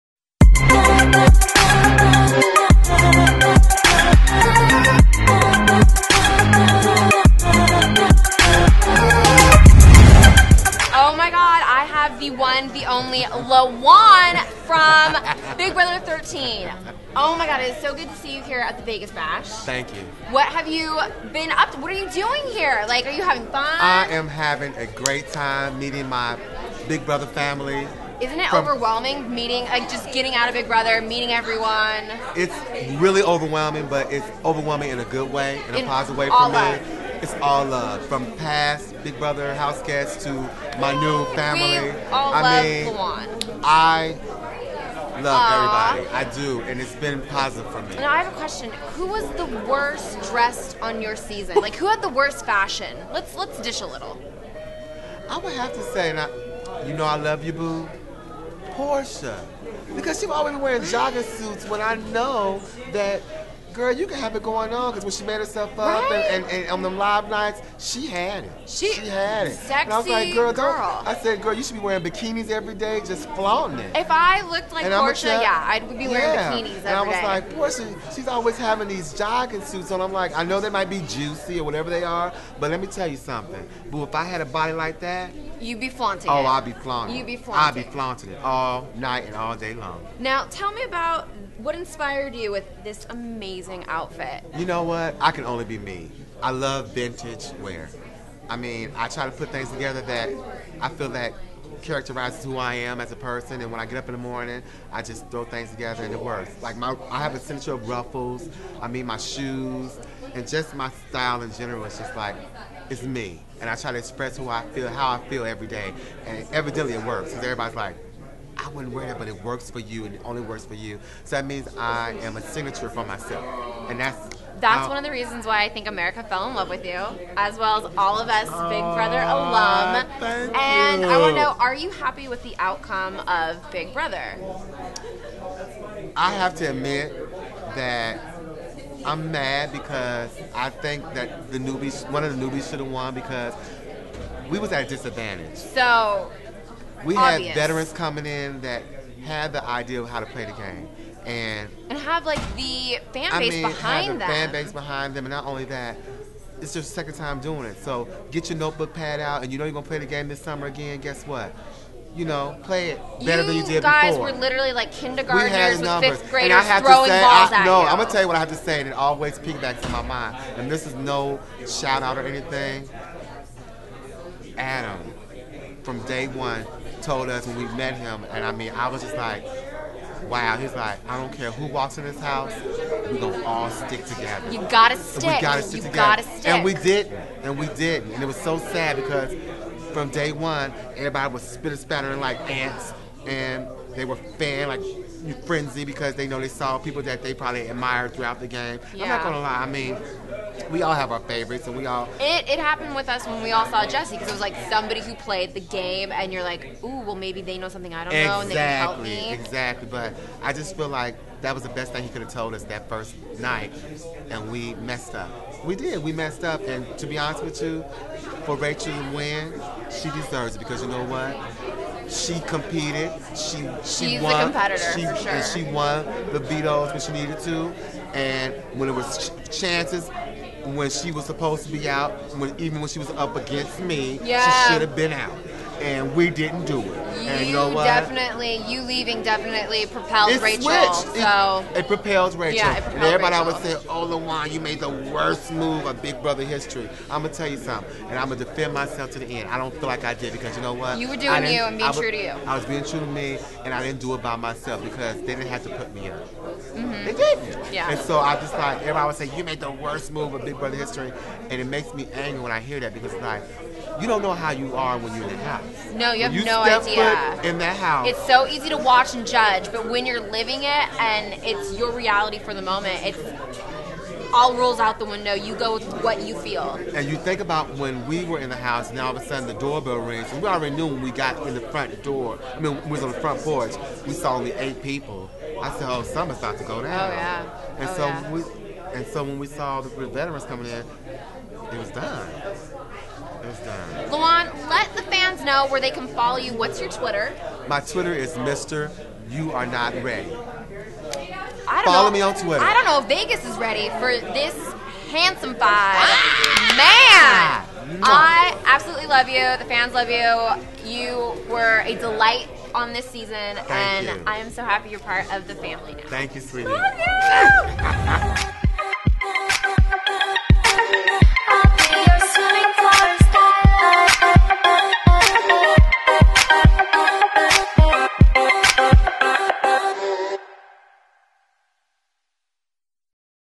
PopTalk Webshow @ Vegas Bash 2011: Lawon Big Brother 13
Kevin Campbell, Lydia Tavera and Chima Simone interview various reality stars from CBS Big Brother and Survivor at the Vegas Bash 2011.

TELL YOUR FRIENDS TO WATCH!!